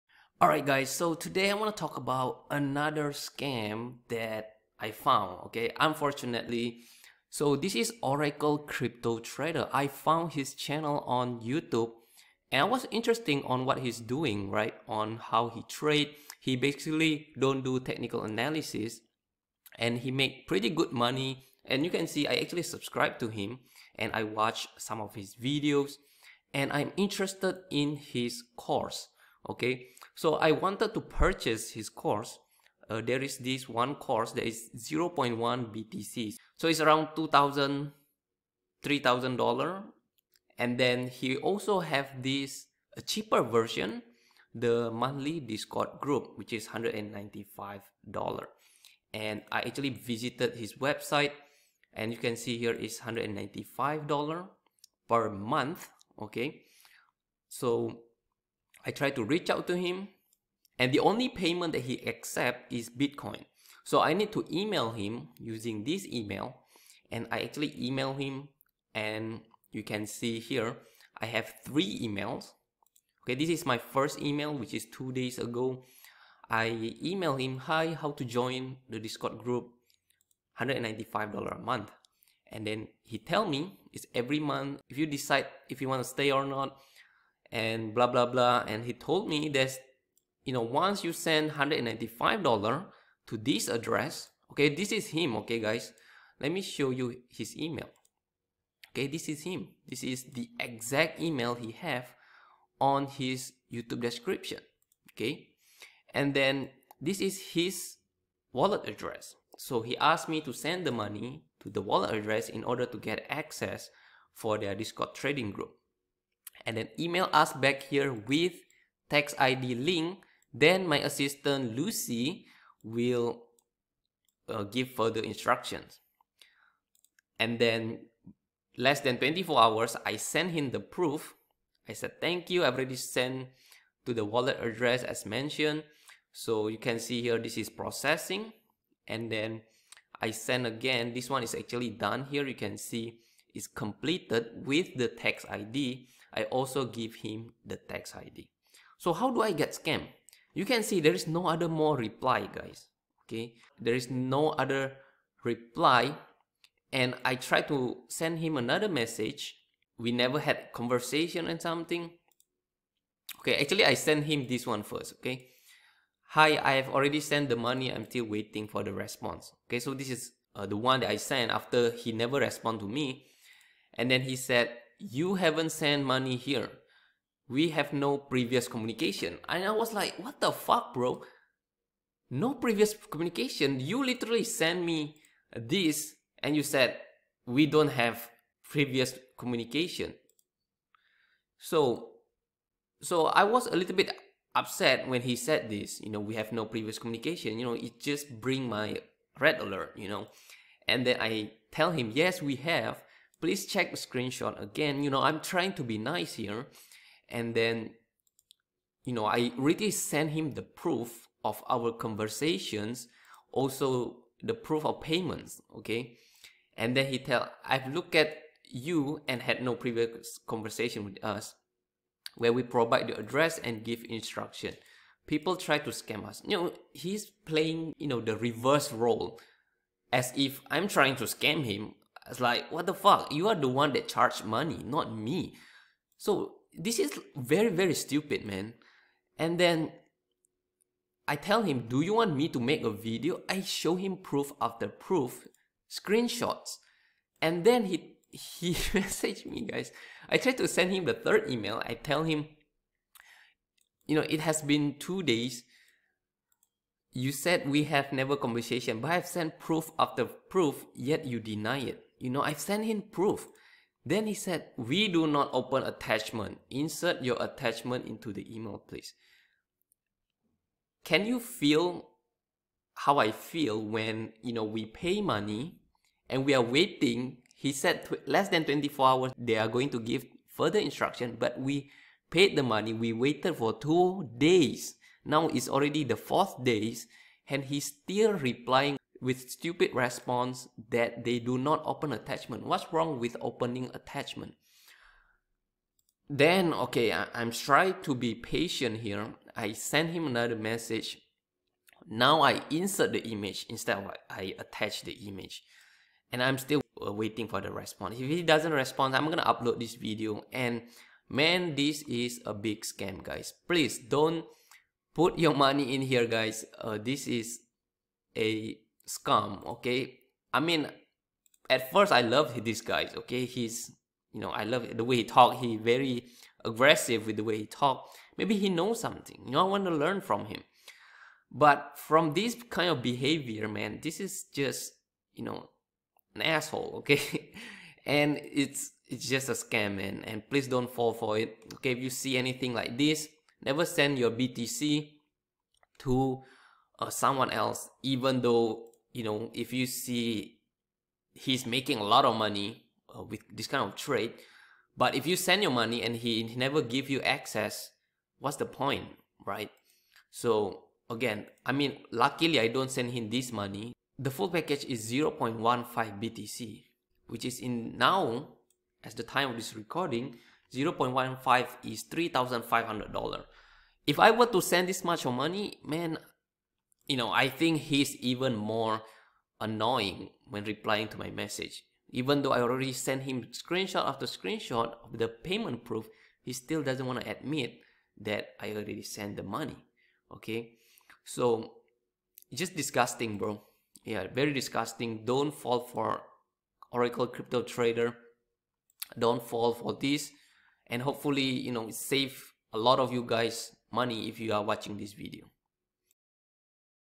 Oracle Crypto Trader is a Scammer! - OMAX Crypto Review
Be careful with ORACLE CRYPTO TRADER. I got scammed after sending BTC to their wallet address. NEver ever you send your hard earned money to this guy. He's a fraud.

Omax Crypto is promising you big profit by trading without using Technical Analysis. He uses platforms like MArgex and Fybit. But no matter how enticing the offer is, if he doesn't live up to the promises. It is just a mere deceit.